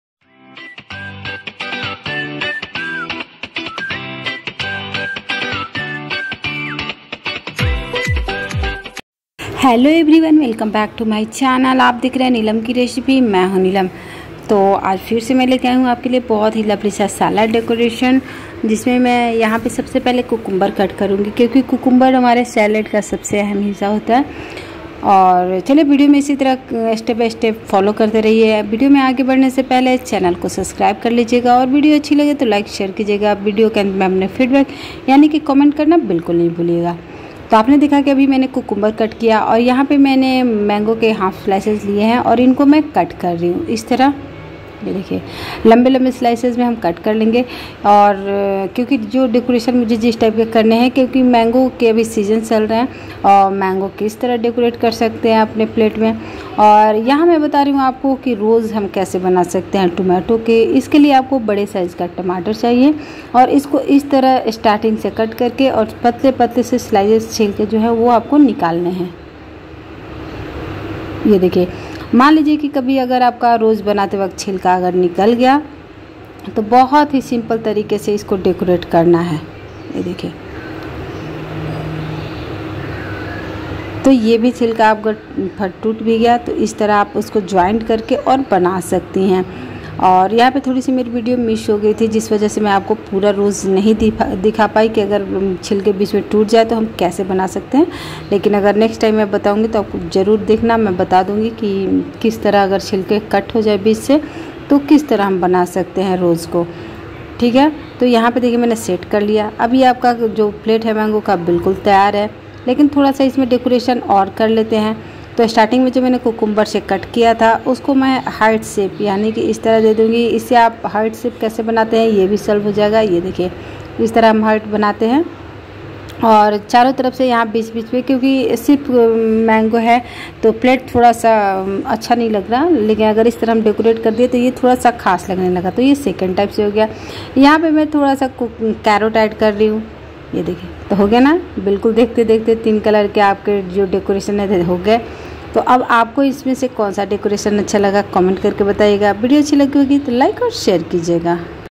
4 Easy Mango Cutting And Decorations ideas By Neelam ki recipes
4 Easy Mango Cutting And Decorations ideas By Neelam ki recipes